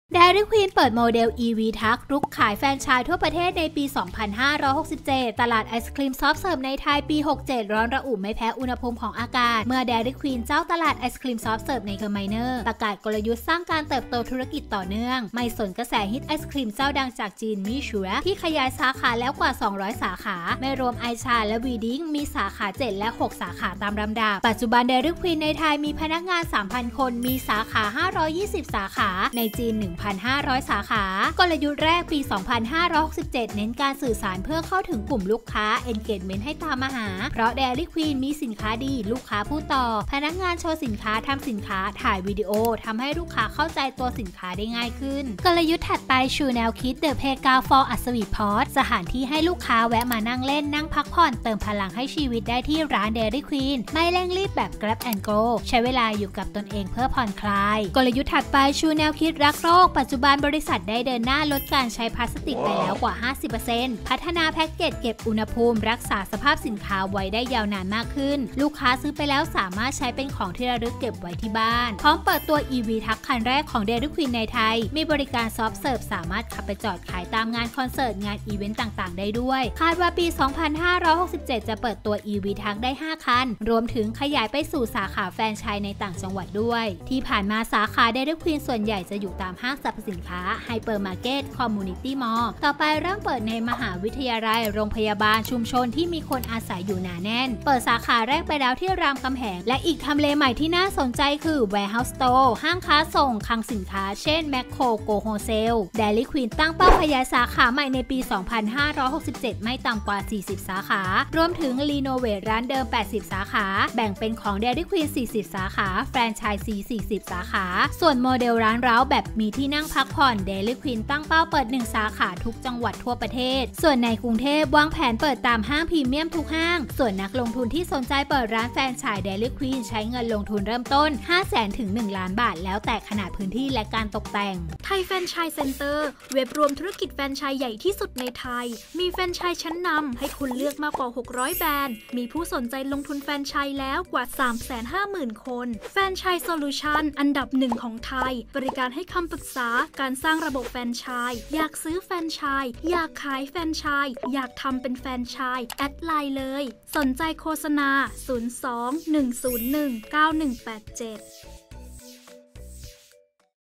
DQ Dairy Queen เปิดโมเดล EV Truck รุกขายแฟรนไชส์ทั่วประเทศในปี 2567
ตลาดไอศกรีมซอฟท์เสิร์ฟในไทยปี 67 ร้อนระอุไม่แพ้อุณหภูมิของอากาศ เมื่อ "แดรี่ควีน" เจ้าตลาดไอศกรีมซอฟท์เสิร์ฟ ในเครือไมเนอร์ฯ ประกาศกลยุทธ์ สร้างการเติบโตธุรกิจต่อเนื่อง คลิก!

ติดต่องานสปอนเซอร์ รีวิวธุรกิจ SMEs ธุรกิจแฟรนไชส์ ผลิตวีดีโอโฆษณา อีเว้นท์

สนใจลงโฆษณา